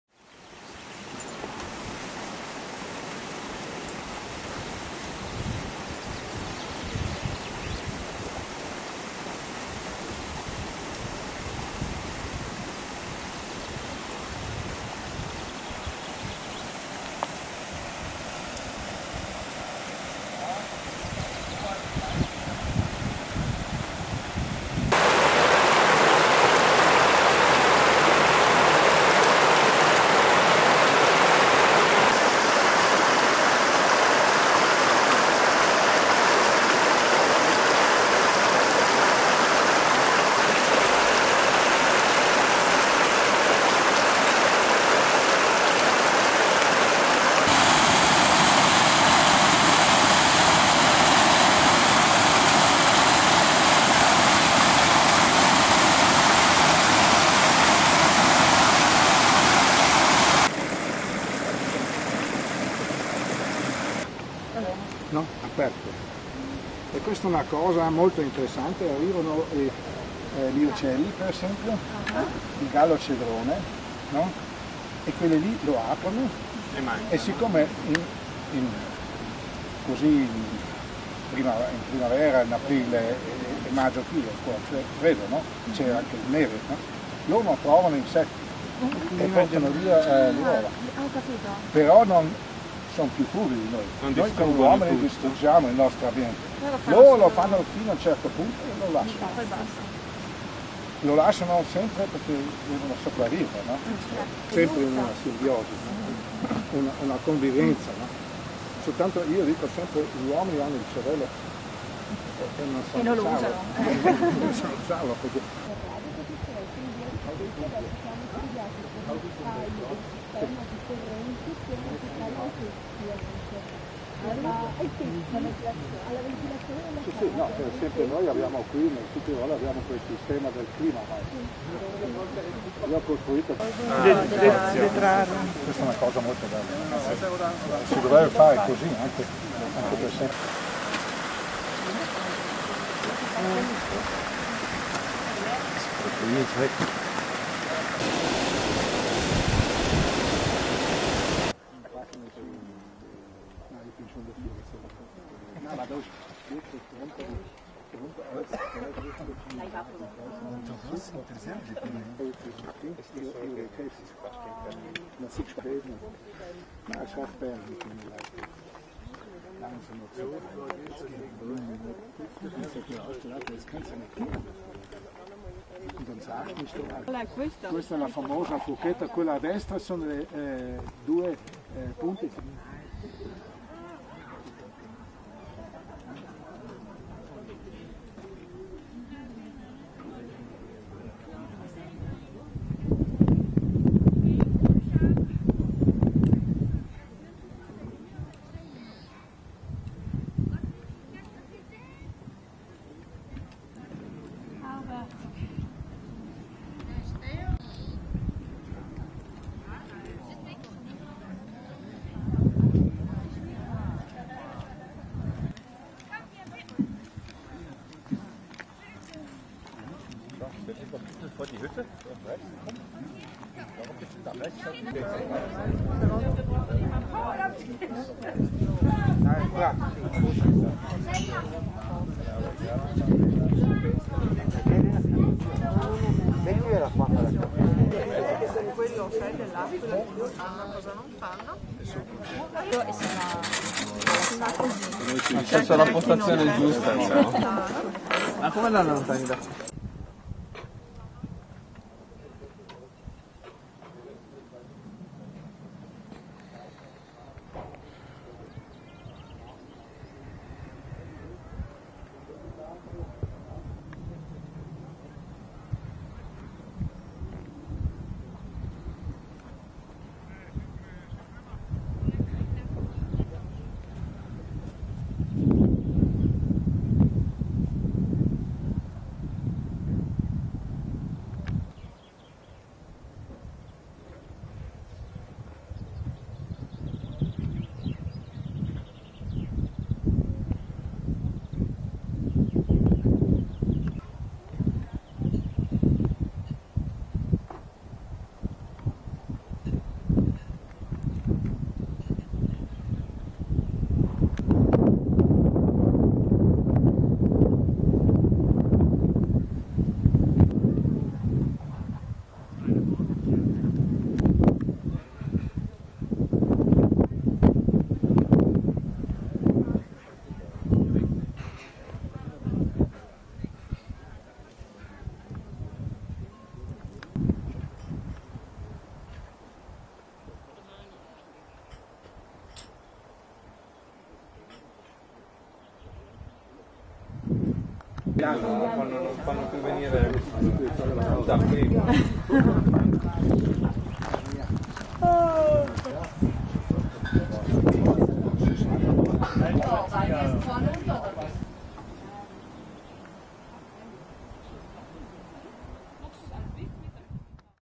Alto Adige Parco naturale di Puez-Odle.mp4
Il Parco naturale di Puez-Odle, in Alto Adige, è un posto meraviglioso: boschi, ruscelli, sentieri di montagna. Un piacere per gli occhi, le orecchie, le gambe e i polmoni.